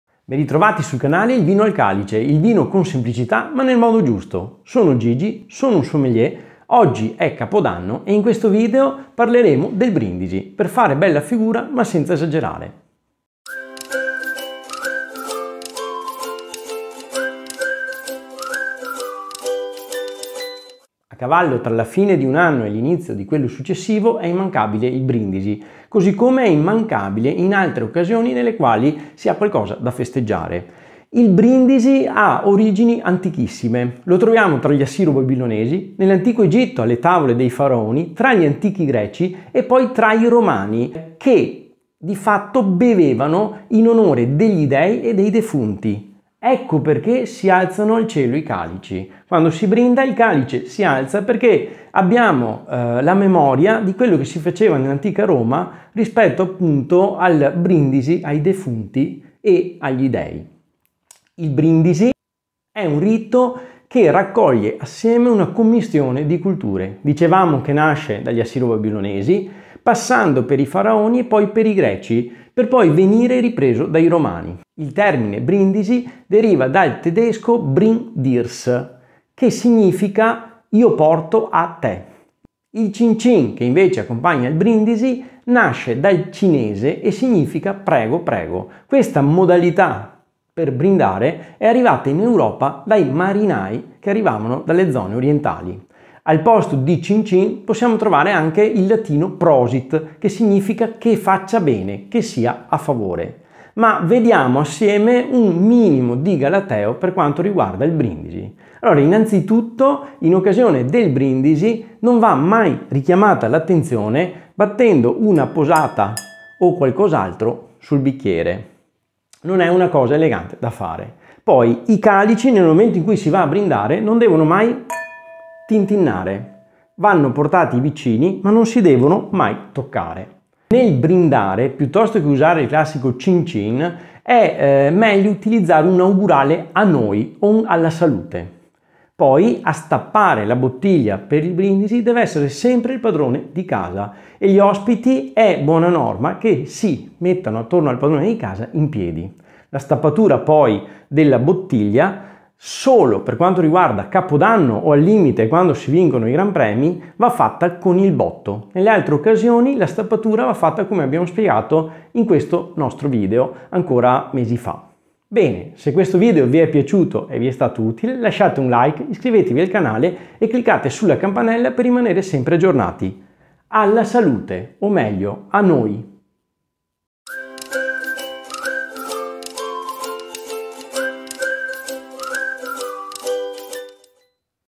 Brindare nel modo giusto
A cavallo tra la fine di un anno e di quello successivo è immancabile il BRINDISI.

Il brindisi ha origini antichissime, lo troviamo tra gli assiro-babilonesi, nell’antico Egitto,  tra gli antichi greci e poi tra i romani quando si beveva in onore degli dei e ai defunti. Ecco perchè si alzano i calici al cielo.
Queste ed altre informazioni troverete in questo video.

Alla Salute!